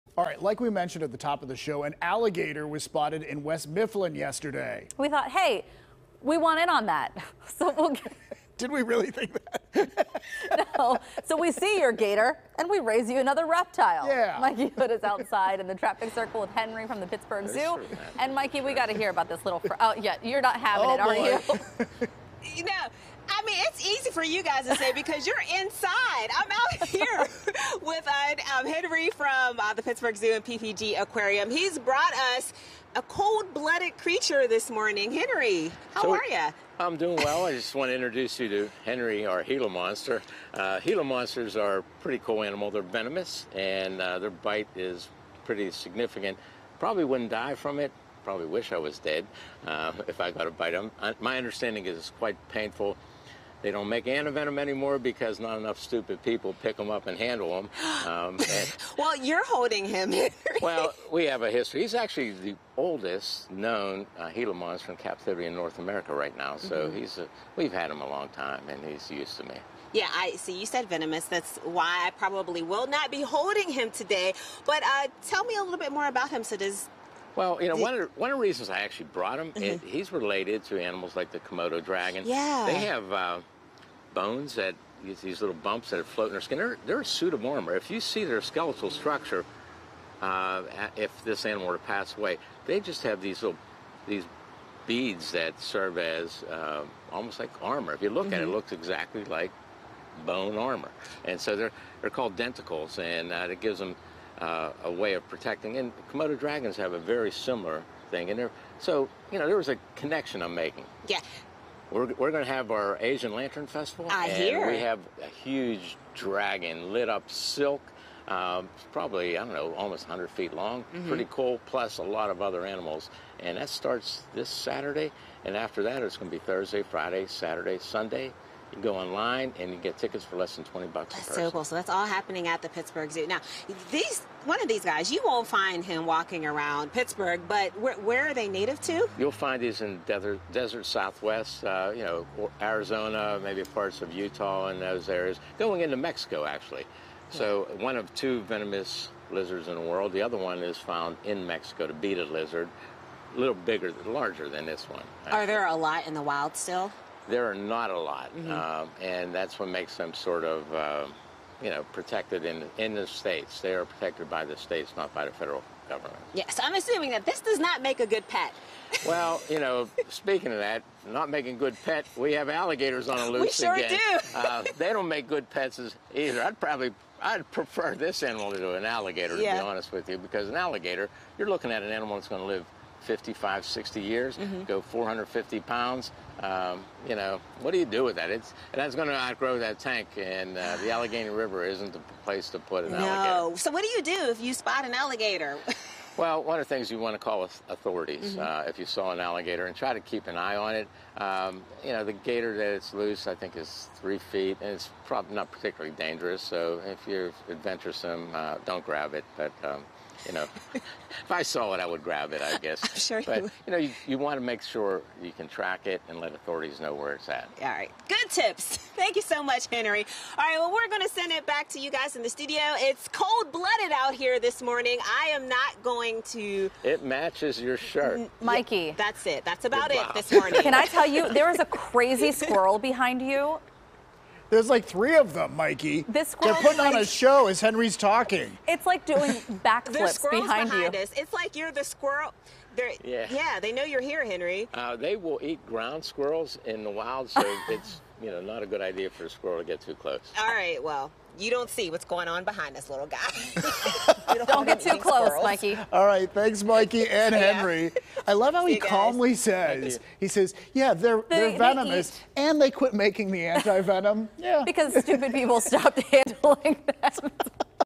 Henry From The Pittsburgh Zoo Previews The Asian Lantern Festival
Pittsburgh Today Live's Mikey Hood visits with Henry Kacprzyk and his friend the Gila Monster from the Pittsburgh Zoo & PPG Aquarium. They are preview the Asian Lantern Festival!